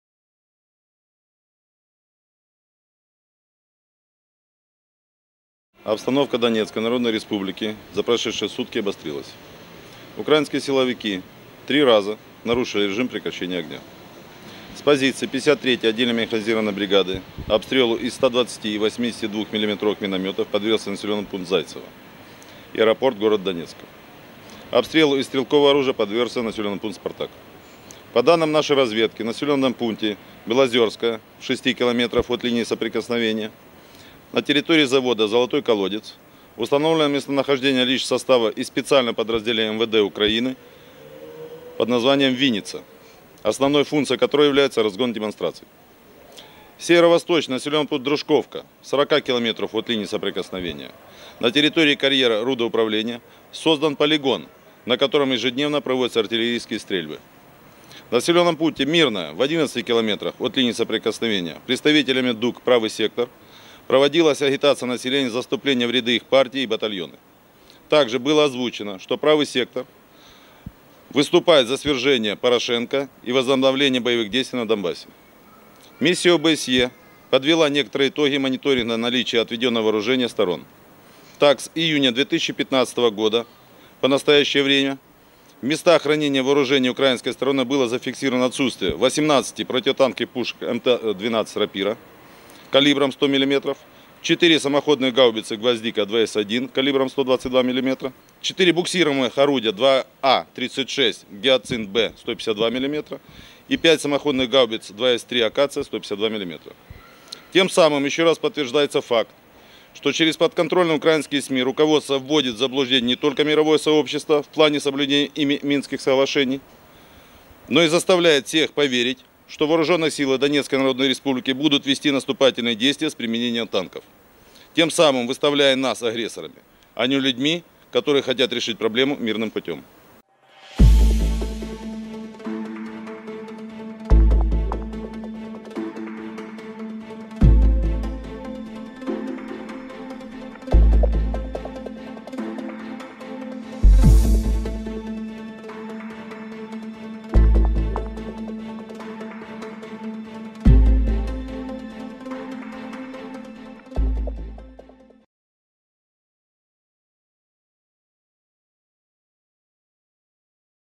Informe del Ministerio de Defensa de la RPD-6 de Octubre 2015 (subtitulos en español)
Informe del Comandante subrogante del cuerpo del Ministerio de Defensa de la RPD-Edward Basurin-6 de Octubre 2015.